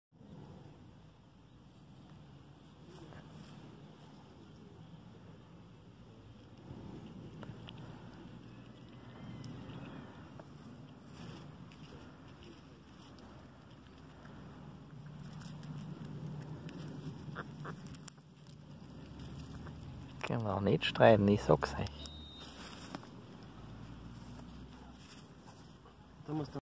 Ground squirrels eating food from my hand
On one of my wildlife trips in Vienna i fed some ground squirrels with nuts.